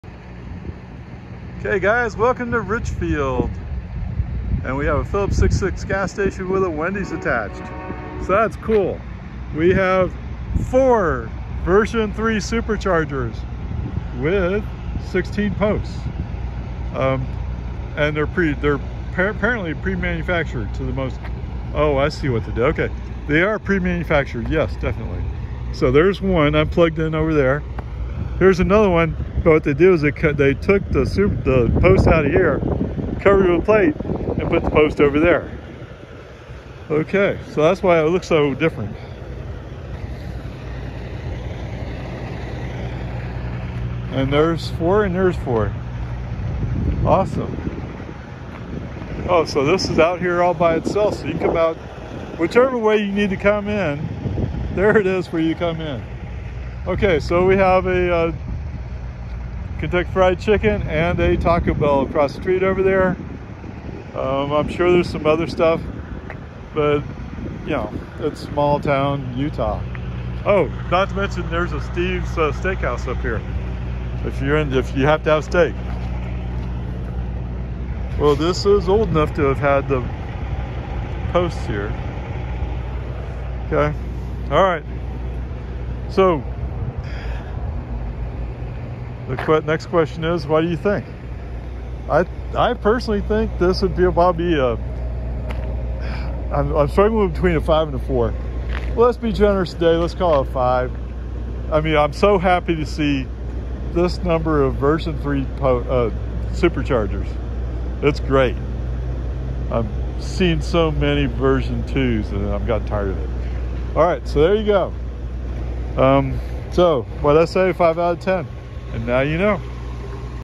Richfield UT SC
A visit to the Tesla Supercharger in Richfield Utah (West 1250 South).

4 V3 Superchargers with 16 posts.
NACS